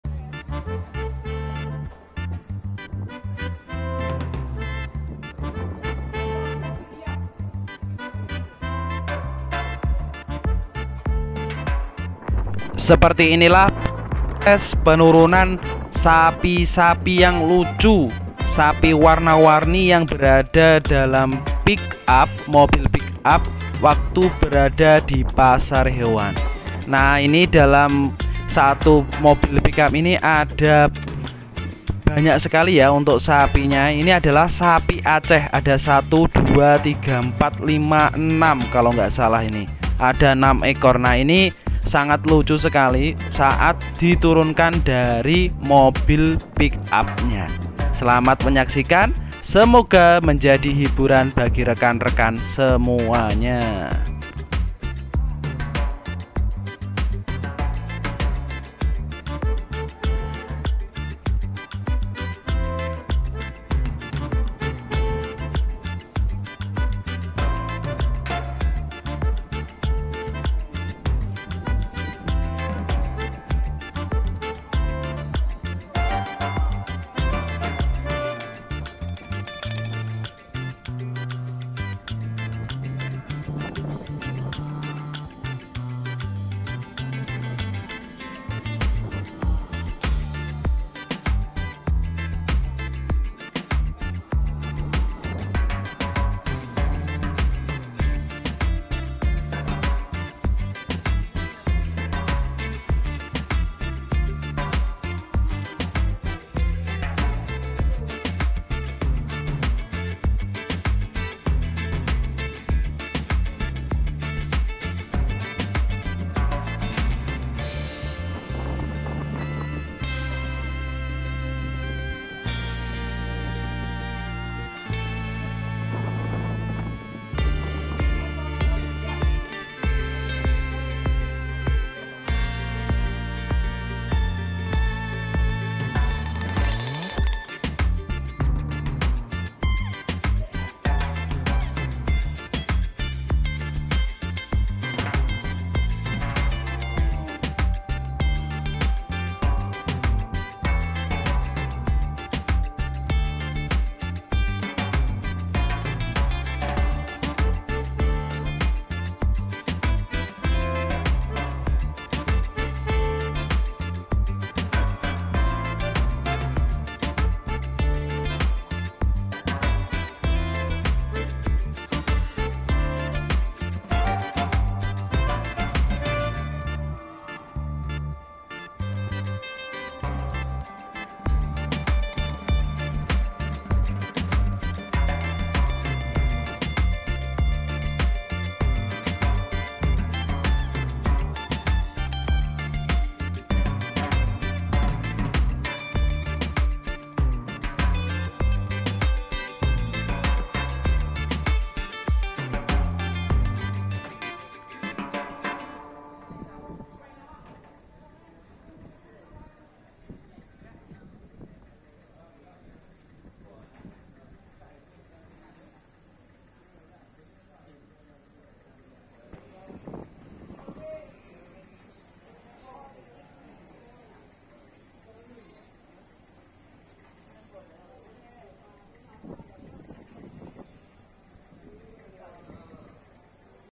Proses Penurunan Sapi Sapi Lucu, Sapi Joget, Sapi Moo, Lembu dari Mobil Pick Oleng di Pasar Hewan
Proses menurunkan sapi sapi dari mobil pick up oleng